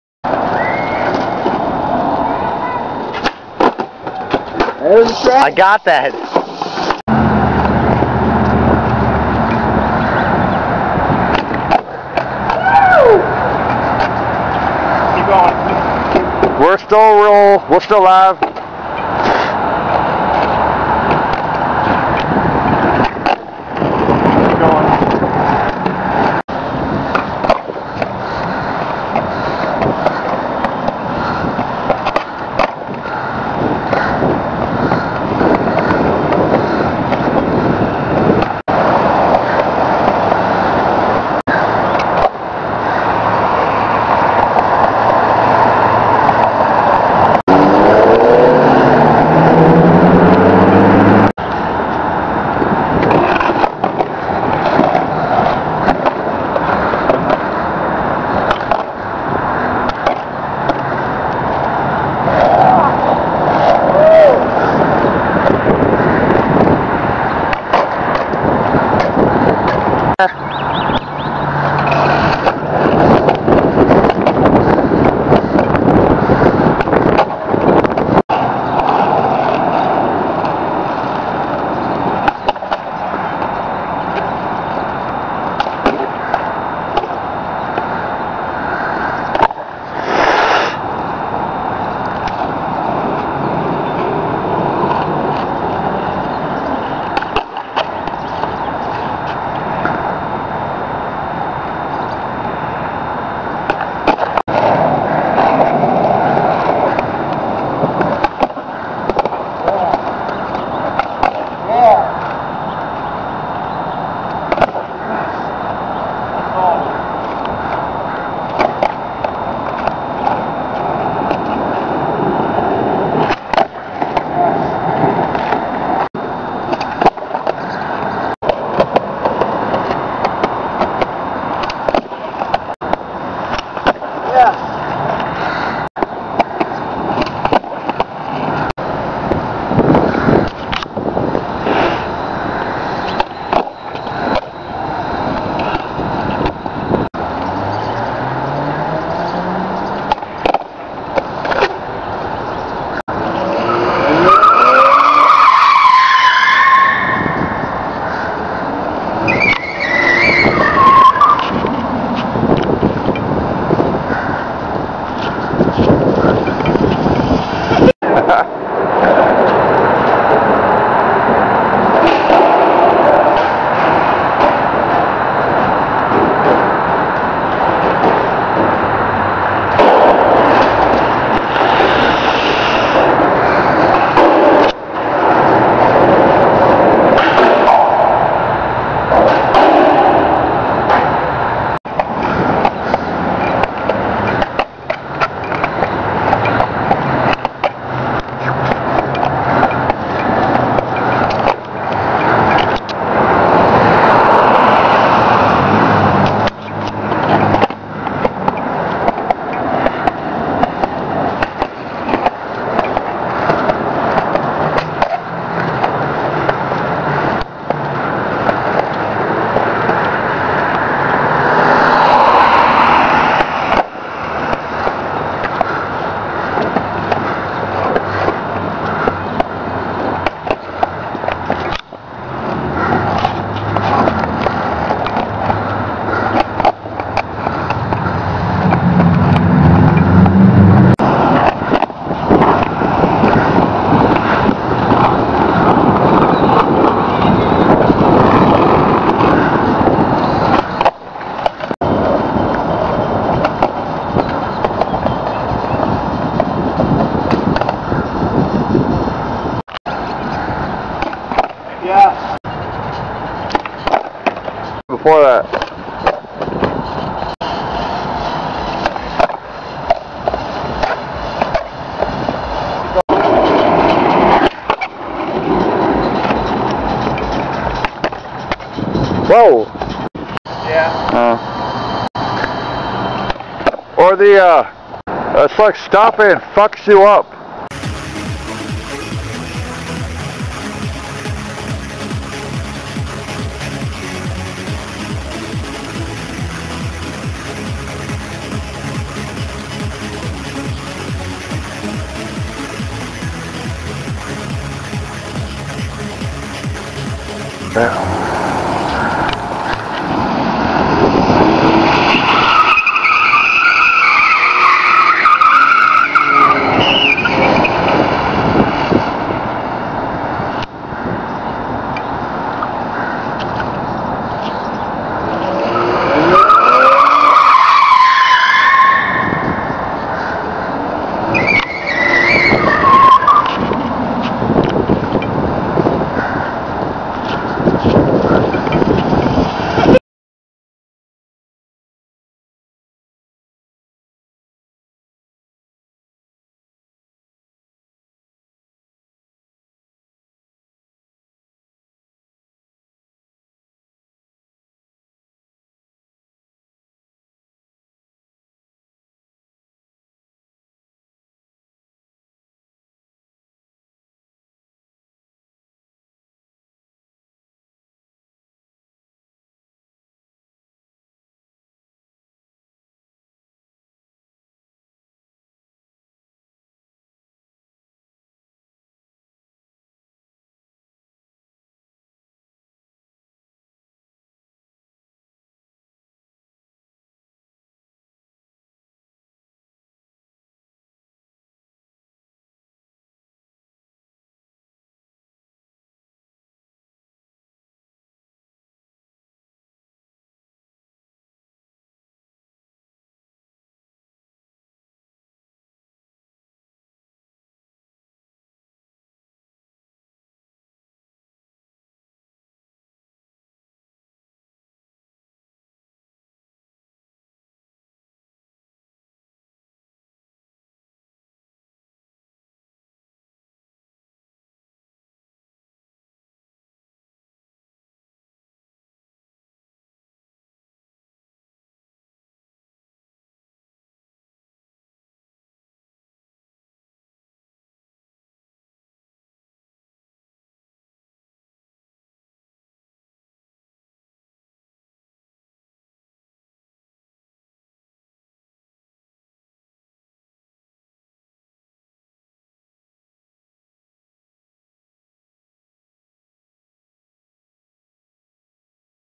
Minneapolis Session
Skateboarding in Minneapolis/Bloomington area